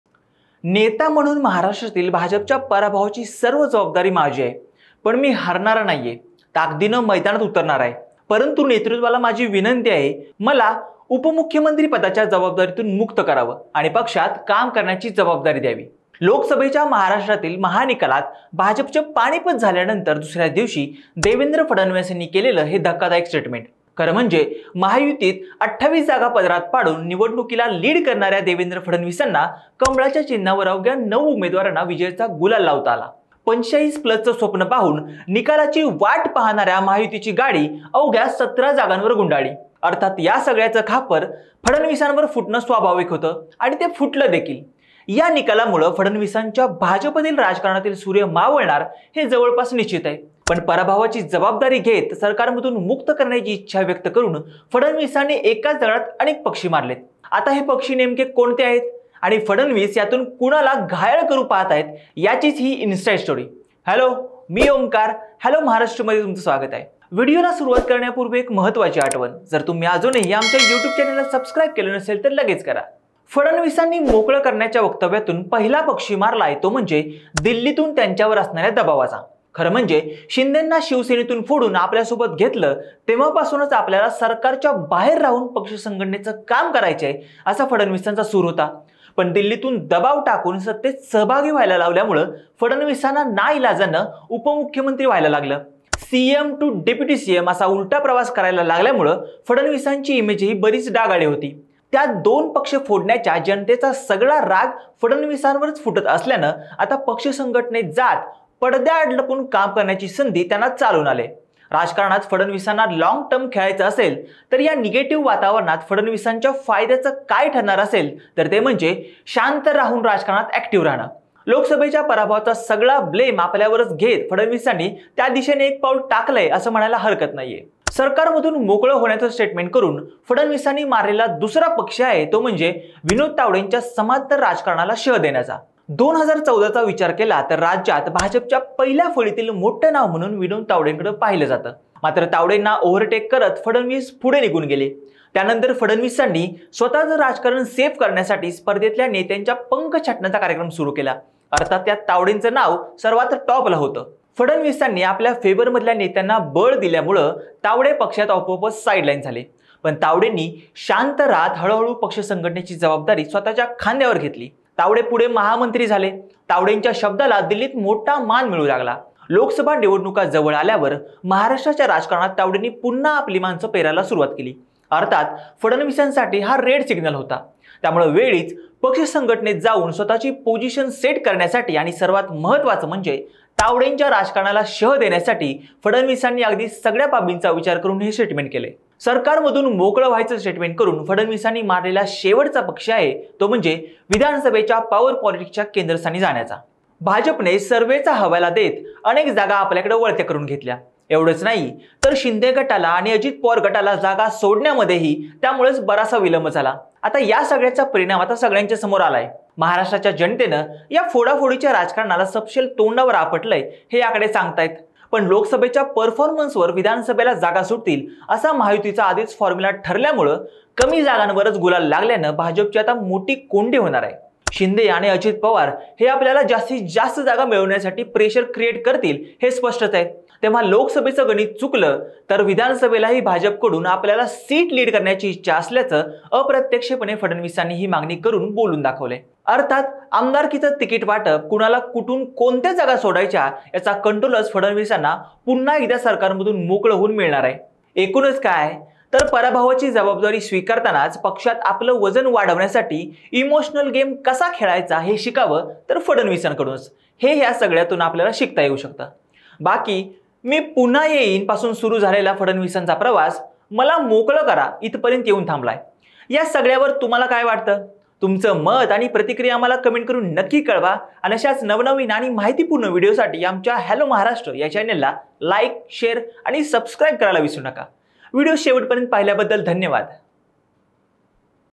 उपमुख्यमंत्रीपदापासून मोकळं करण्याचं वक्तव्य, Devendra Fadanvis यांची चाल | Maharashtra Lok Sabha
नेता म्हणून महाराष्ट्रातील भाजपच्या पराभवाची सर्व जबाबदारी माझी आहे. पण मी हरणारा नाही, पण ताकदीने मैदानात उतरणार आहे. परंतु नेतृत्वाला माझी विनंती आहे, मला उपमुख्यमंत्रीपदाच्या जबाबदारीतून मुक्त करावे आणि पक्षात काम करण्याची जबाबदारी द्यावी…लोकसभेच्या महाराष्ट्राच्या महानिकालात भाजपचे पानिपत झाल्यानंतर दुसऱ्याच दिवशी देवेंद्र फडणवीसांनी केलेलं हे धक्कादायक स्टेटमेंट… खरं म्हणजे महायुतीत 28 जागा पदरात पाडून निवडणुकीला लीड करणाऱ्या देवेंद्र फडणवीसांना कमळाच्या चिन्हावर अवघ्या नऊ उमेदवारांना विजयाचा गुलाल लावता आला…45 प्लसचं स्वप्न पाहून निकालाची वाट पाहणाऱ्या महायुतीची गाडी अवघ्या १७ जागांवर गुंडाळली.. अर्थात या सगळ्याचं खापर फडणवीसांवर फुटणं स्वाभाविक होतं आणि ते फुटलं देखील… या निकालामुळे फडणवीसांच्या भाजपातील राजकारणातील सुर्य मावळणार हे तर जवळजवळ निश्चितय. पण पराभवाची जबाबदारी घेत सरकारमधून मुक्त करण्याची इच्छा व्यक्त करुन फडणवीसांनी एकाच दगडात अनेक पक्षी मारलेत… आता हे पक्षी नेमके कोणते आहेत? आणि फडणवीस यातून कुणाला घायाल करु पाहतायत? त्याचीच ही इनसाईड स्टोरी…

As a leader, I bear full responsibility for BJP's defeat in Maharashtra. But I am not a loser; I will enter the field with renewed strength. However, I request the leadership to relieve me from the responsibility of Deputy Chief Minister and allow me to focus on party work. This shocking statement from Devendra Fadnavis came on the second day after BJP's defeat in the Maharashtra Assembly of the Lok Sabha. Fadnavis, who led the election and saw the Grand Alliance lose 28 seats, has only nine candidates under the lotus symbol. The anticipated victory turned into a setback as the Grand Alliance, dreaming of 45-plus seats, ended up with just 17. Naturally, the blame fell on Fadnavis. This result almost certainly signals the end of Fadnavis' prominent role in BJP politics. However, by taking responsibility for the defeat and expressing his desire to step away from the government, Fadnavis has made a strategic move. He has targeted several objectives with this statement. Who exactly are these targets? And what is Fadnavis' true intention? This is his inside story.